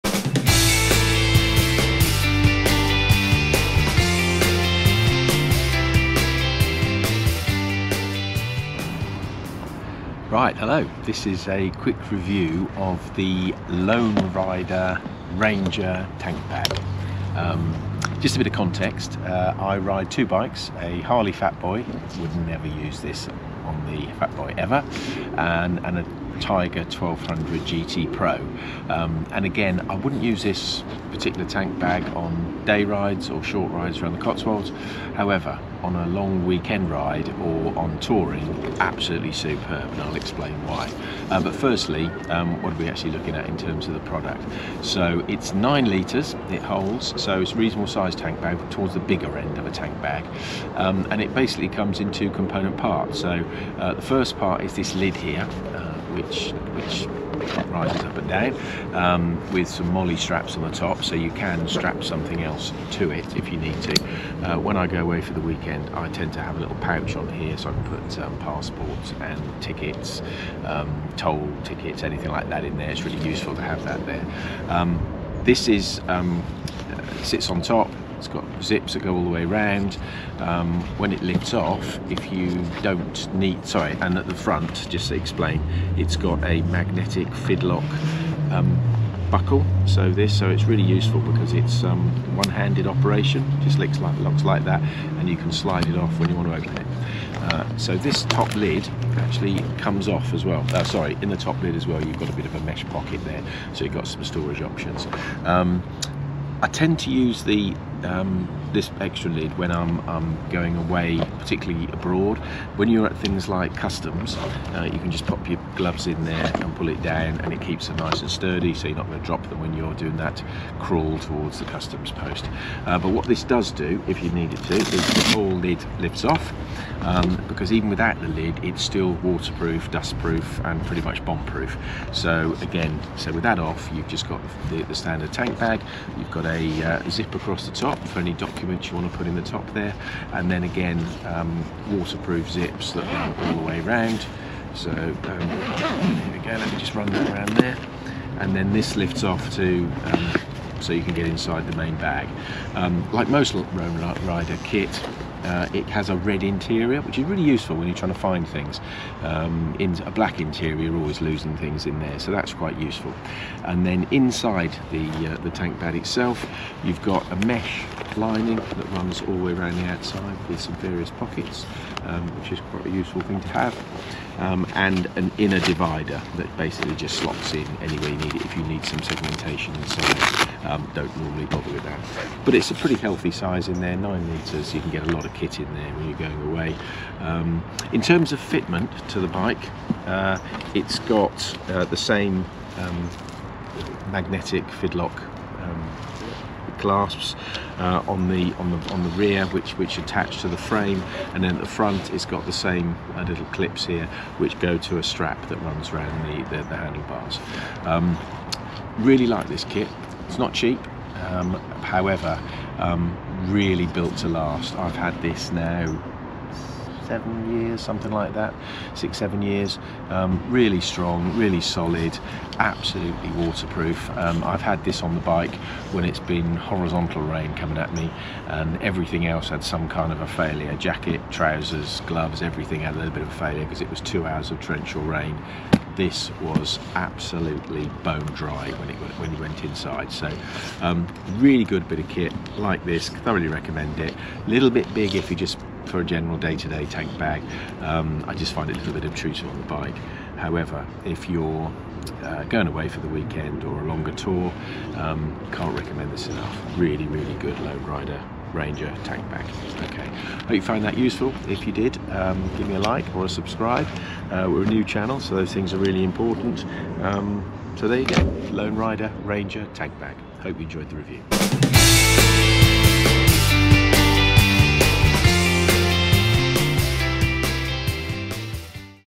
Lone Rider tankbag review 1080WebShareName
A review of the Lone Rider Ranger tank bag. This is a pricey but superb piece of kit that is waterproof, dust proof and pretty much bomb proof.